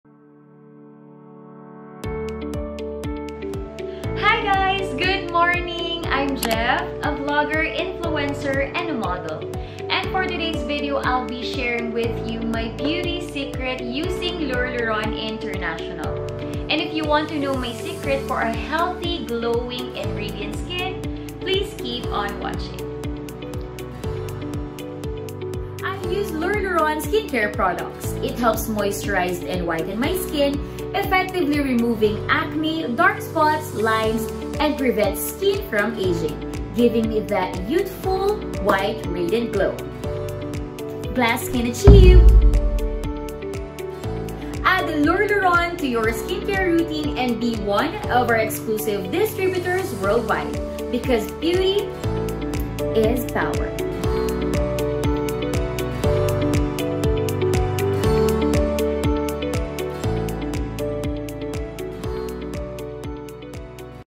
Korean Glass Skin Glow - Jeff Serrano
Dear Lueur Lauren Family,

Watch and discover as Jeff reveals her beauty secret for her signature Glass Skin Glow.